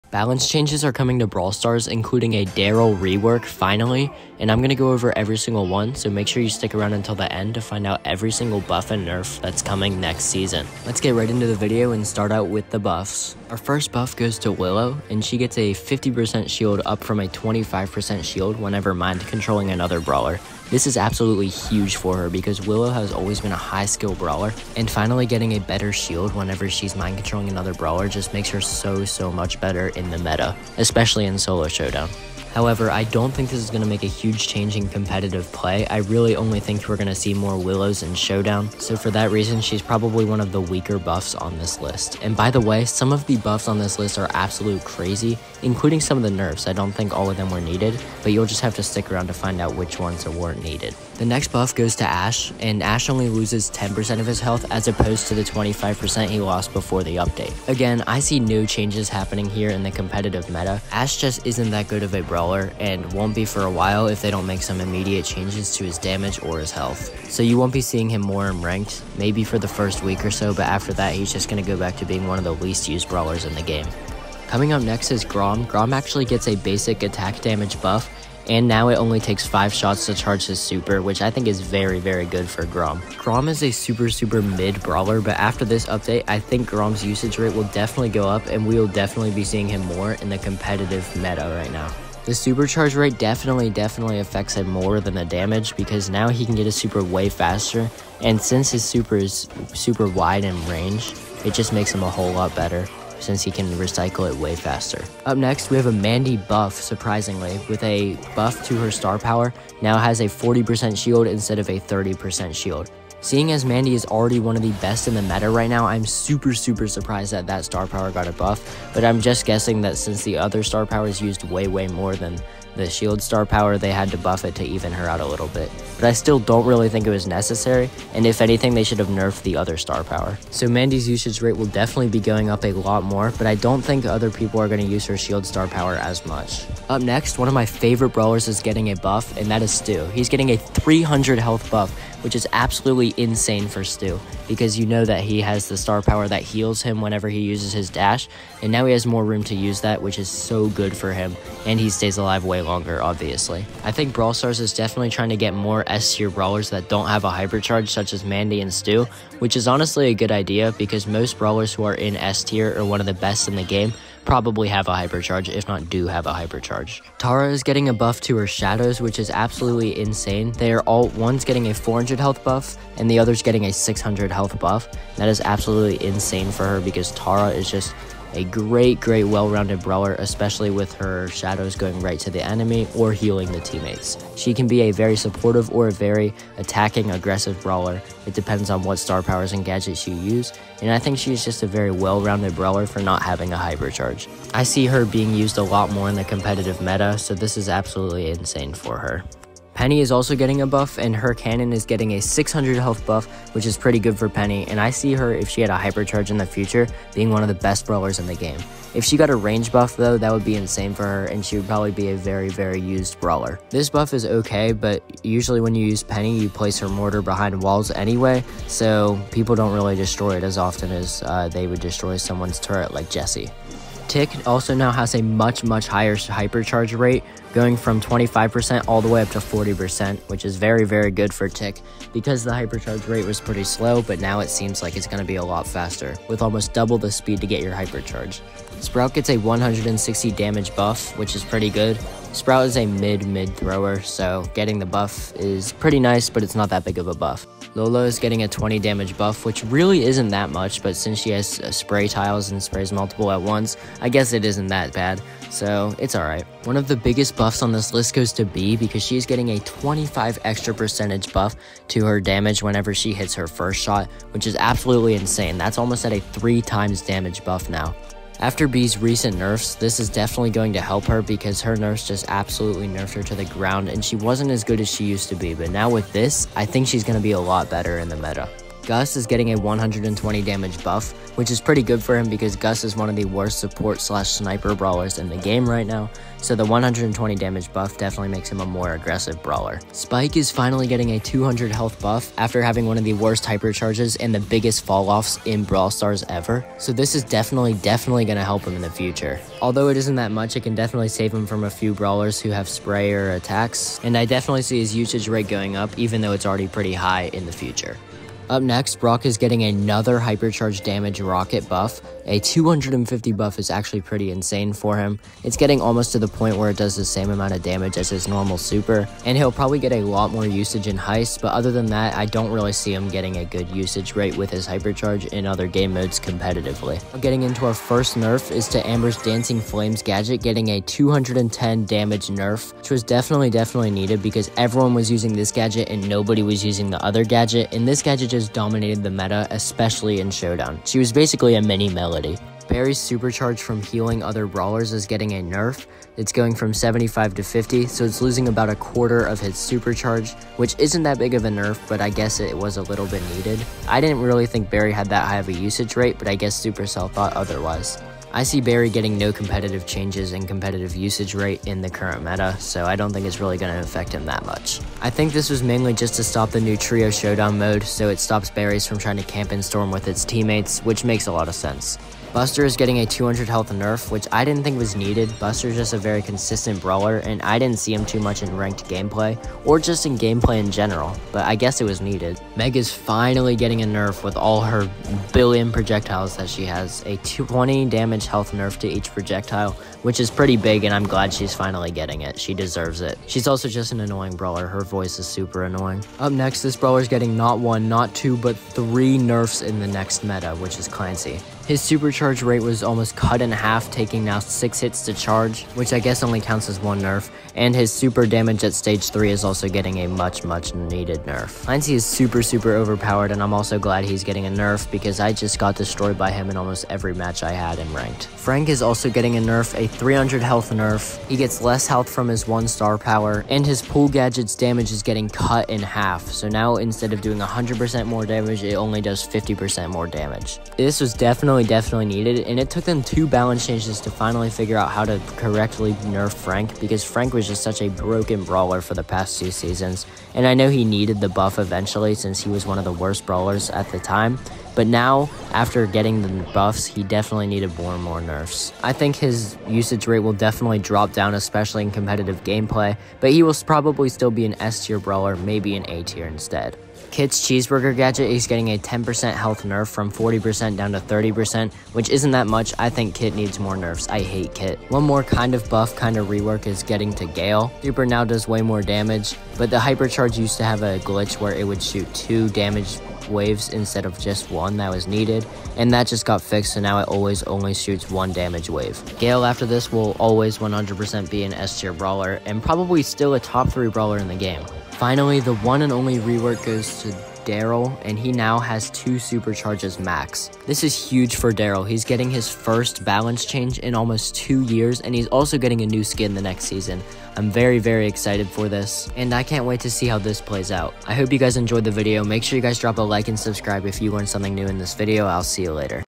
BALANCE CHANGES ARE COMING! (Darryl Rework and Much More!)
Balance Changes are arriving next week in Brawl Stars! I'm going over every single balance change that's coming in brawl stars, the meta will definitely be more versatile and include a lot more brawlers in competitive gameplay.

Credit to AshBS for balance changes